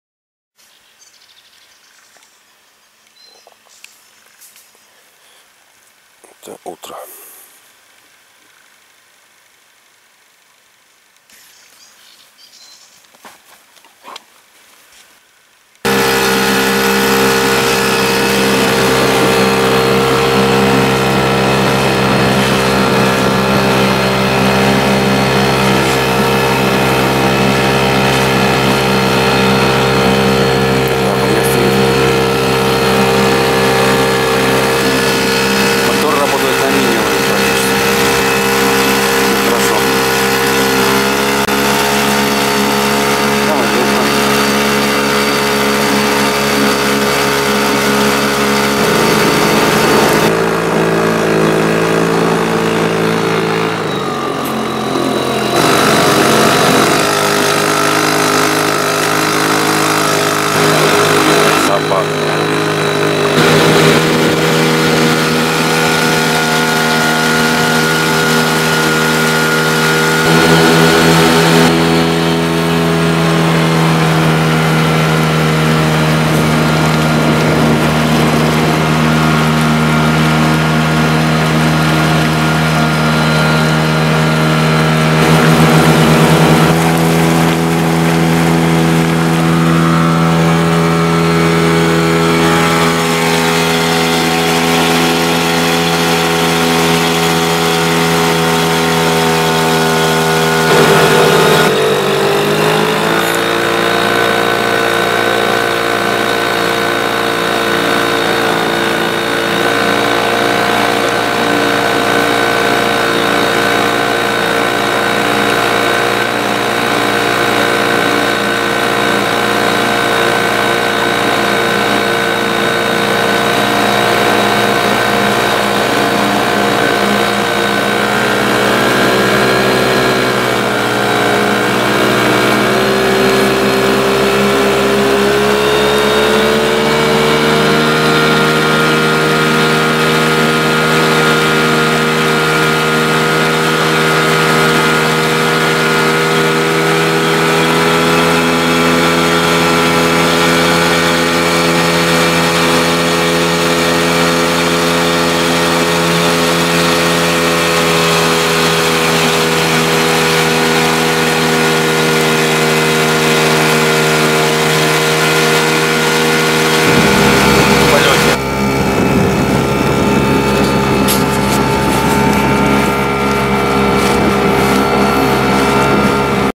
"420 Игла" с Ямаха-3 и "Аква-280" с Ямаха-2
Один из дней отдыха на Клязьминском водохранилище. Обход акватории на лодках с мелкомоторами. В то время у меня был ещё "Флагман 420 Игла".
Тут моторы YAMAHA-3, YAMAGA-2